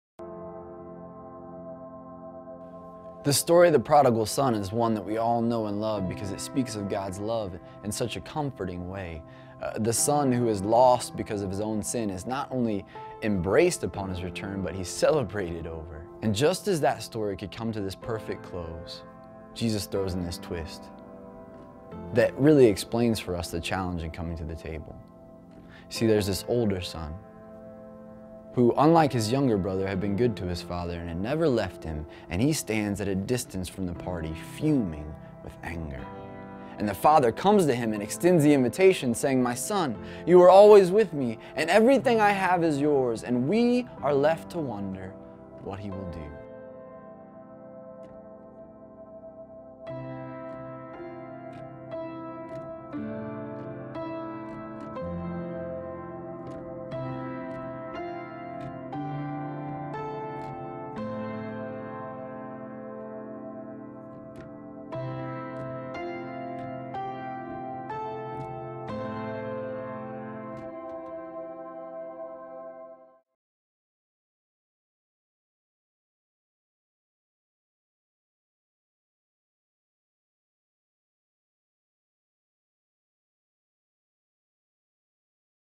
Living Mission: The Table Trailer
Trailer for The Table lesson on Living Mission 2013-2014. This NMI Adult Mission Education resource is available from Nazarene Publishing House.